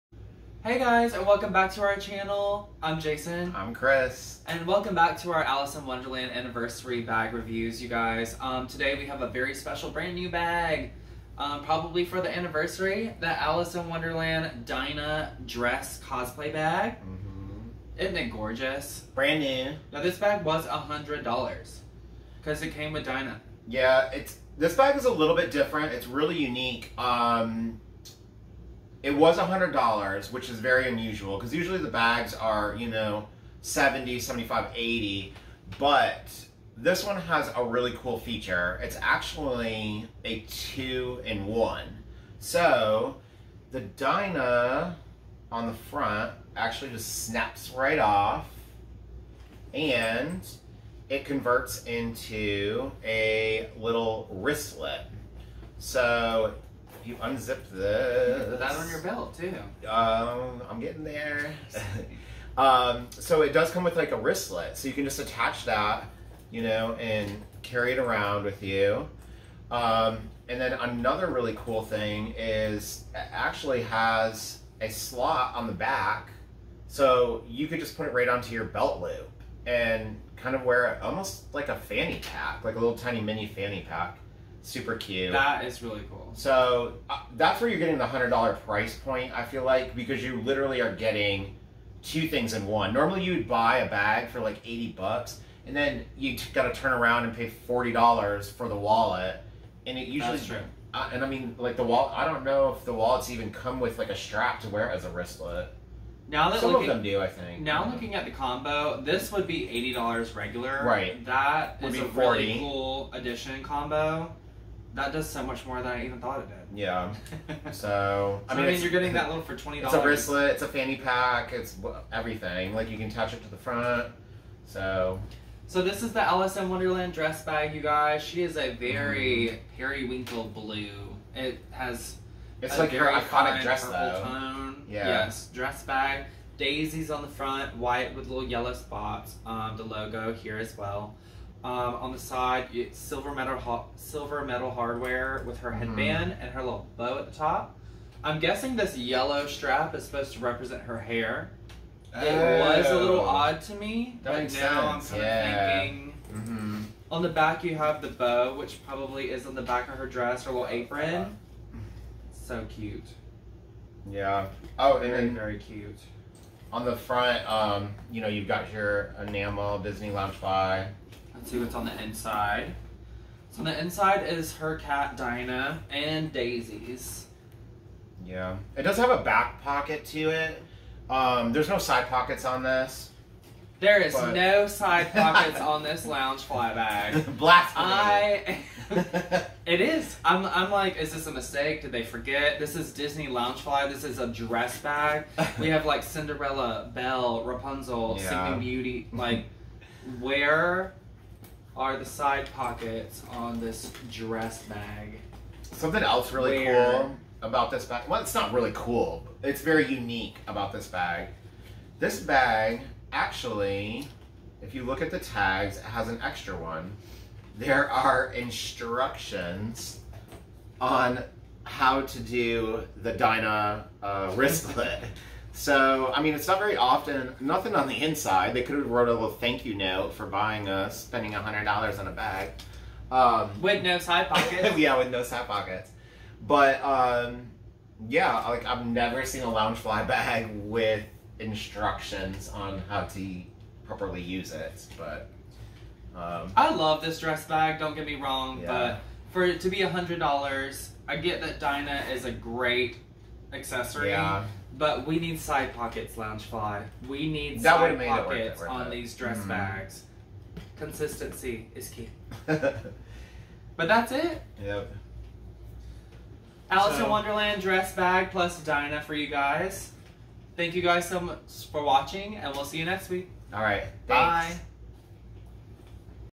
Loungefly X Disney Alice Cosplay Dress Mini Backpack
A 2 for 1! Showing our brand new alice bag!